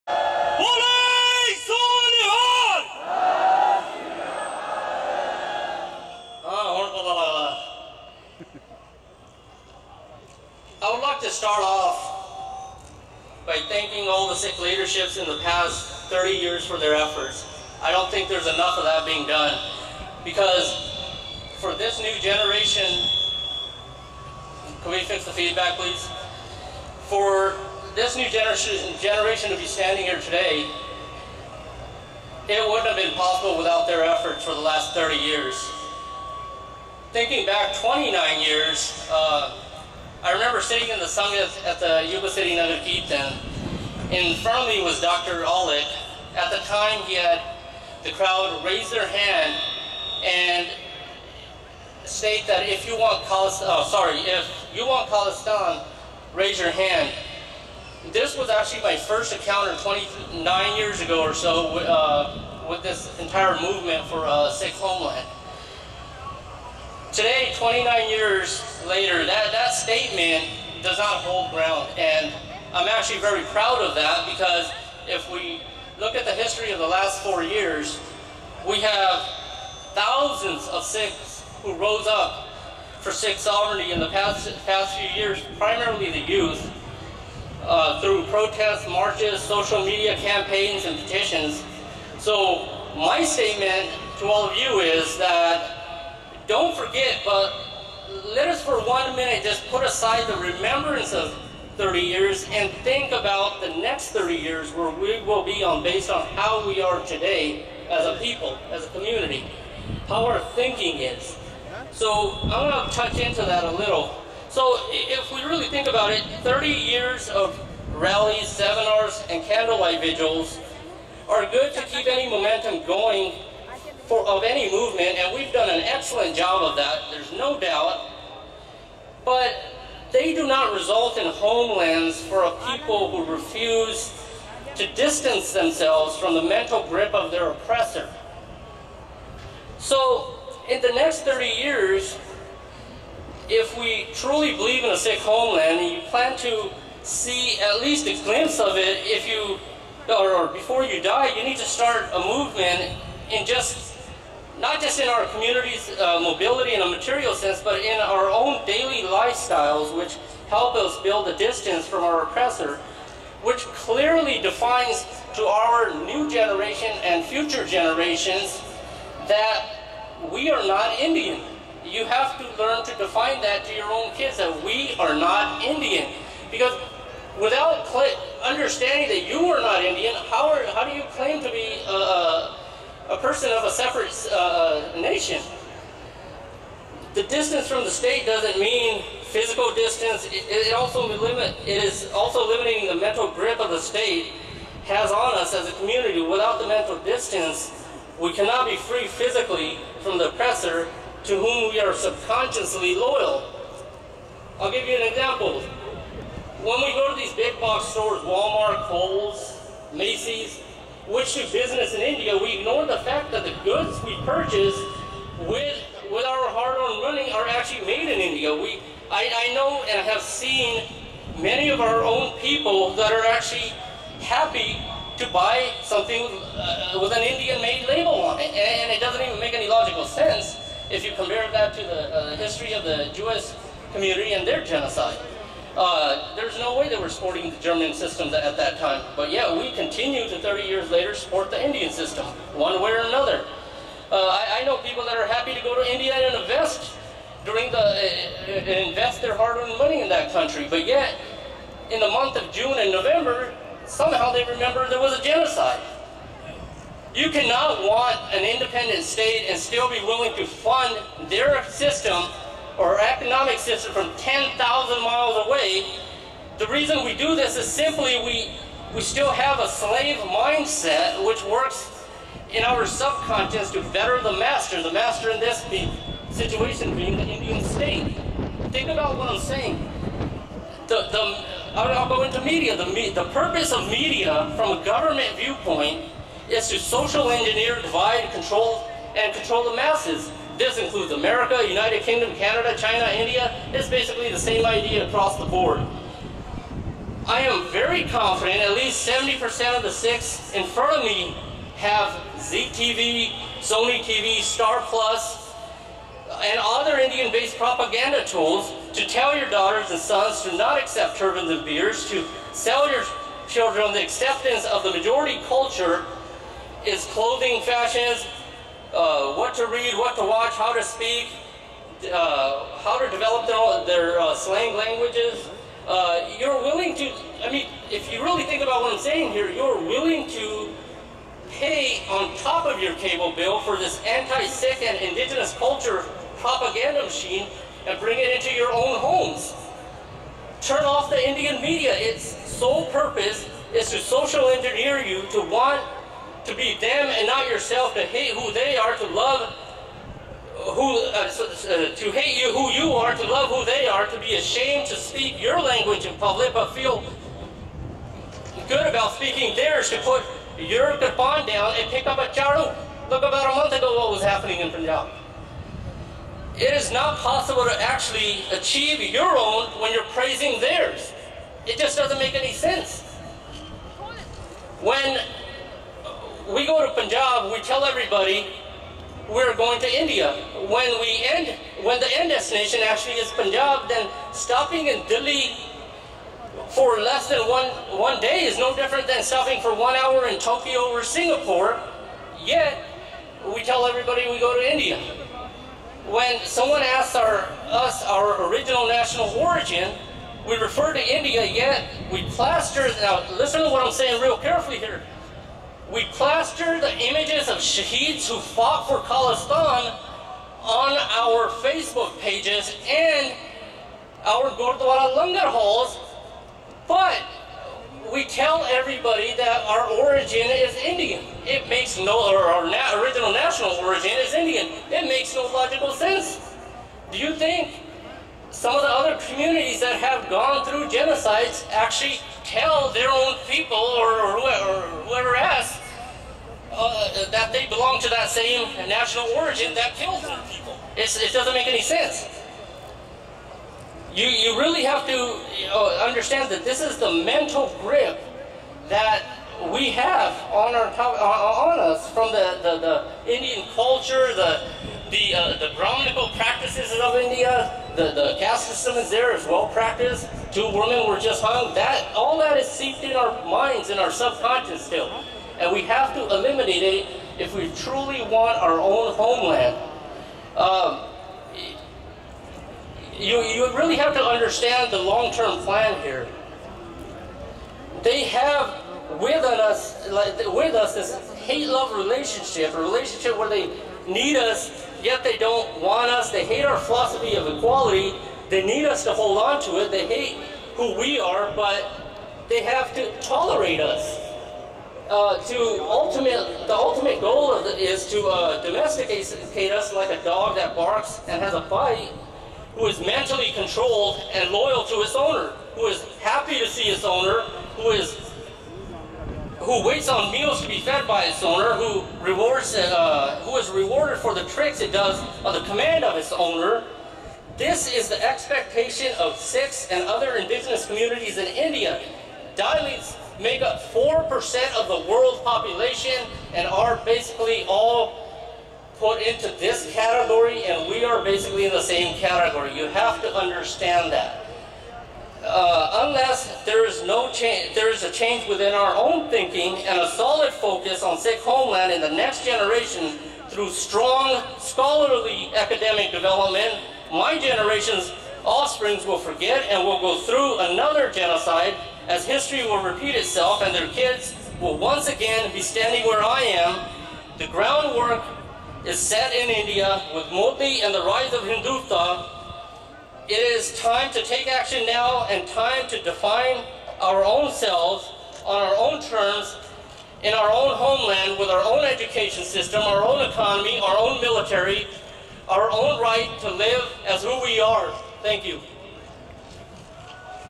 Nagar Kirtan San Francisco Ca 2014 speech 1 (english)
Bhai Tajinder Singh Ji
English